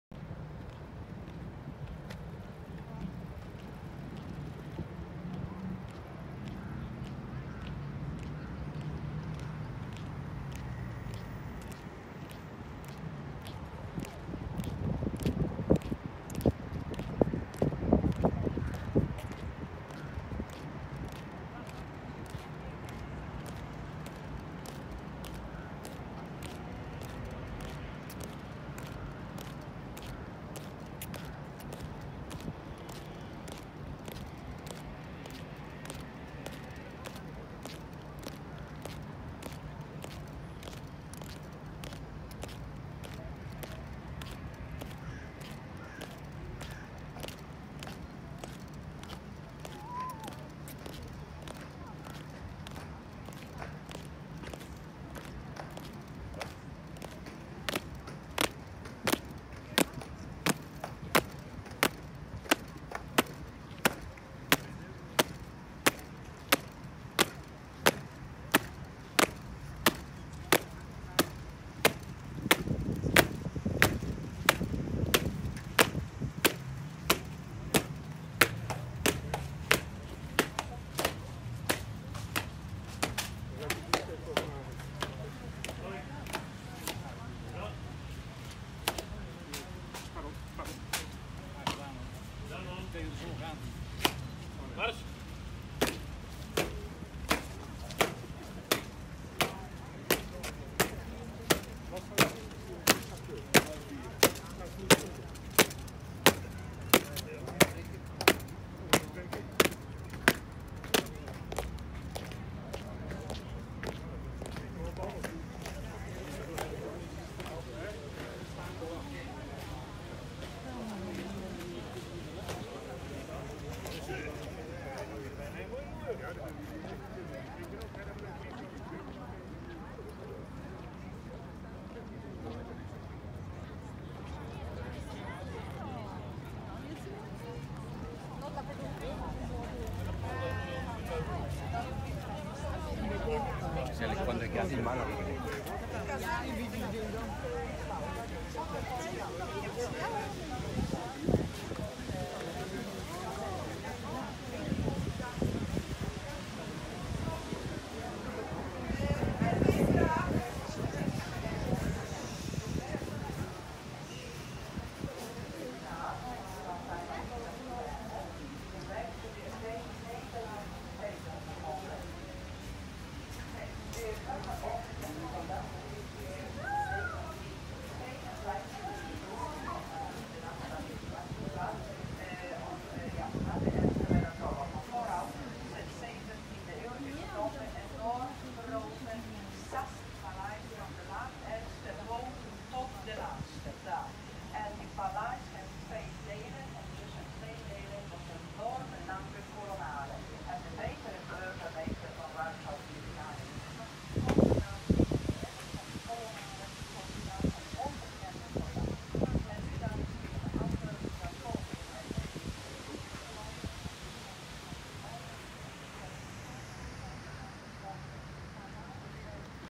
2019 08 06 Zmiana Warty przy Grobie Nieznanego Żołnierza w Warszawie.
Piękna ceremonia, która podtrzymuje pamięć o polskich bohaterach narodowych. Zawsze o nich pamiętajmy. Cześć Ich Pamięci, zginęli na polach bitew broniąc niepodległości Polski. Dziś dla wielu patriotyzm jest absolutnie nie popularny. My Polacy mamy obowiązek podtrzymywać pamięć o naszych bohaterach. To nasz obowiązek.